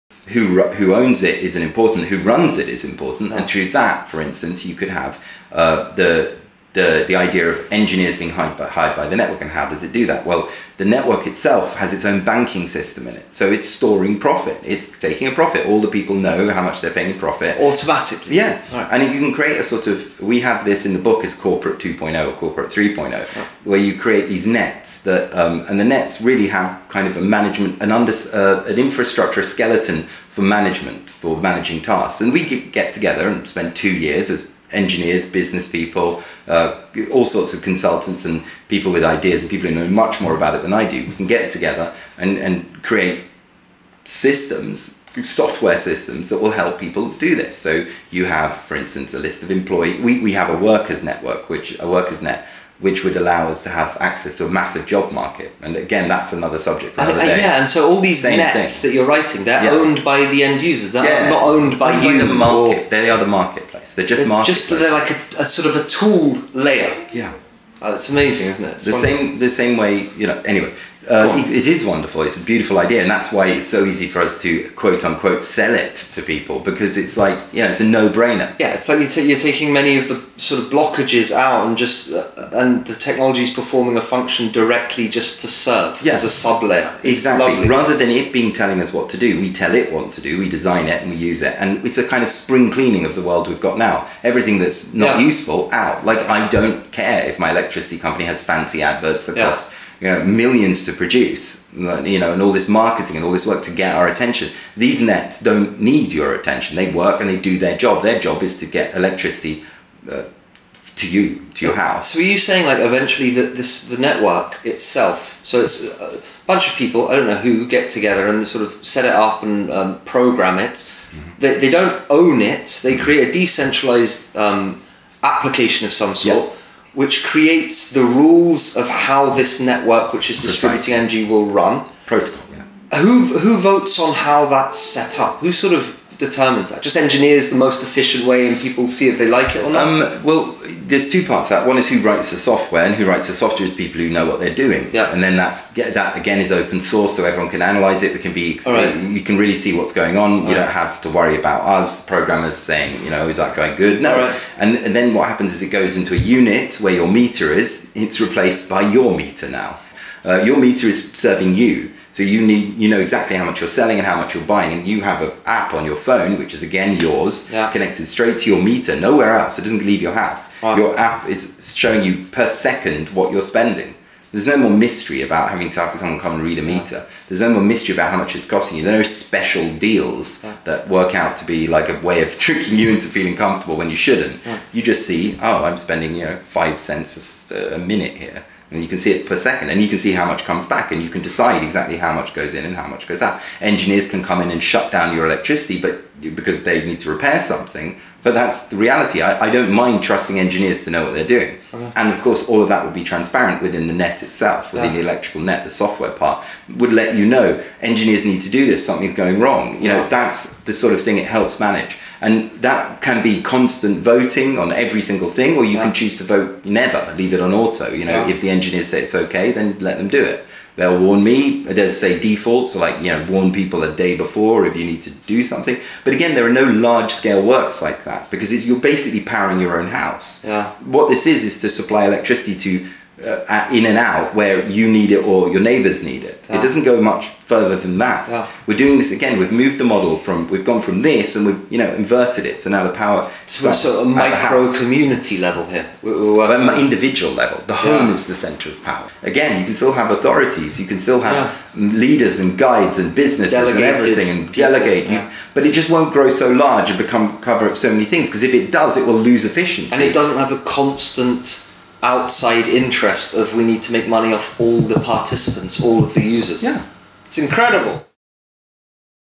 Decentralized Electricity Governance and Management [Clips]
How can a decentralized network actual operate a business with no ownership model? This clip summarizes the idea of soon-to-be present systems that operate much like businesses but completely resist centralized control. Managers, employees and their own banking systems. Future engineering.

Filmed in Malta, June 2019.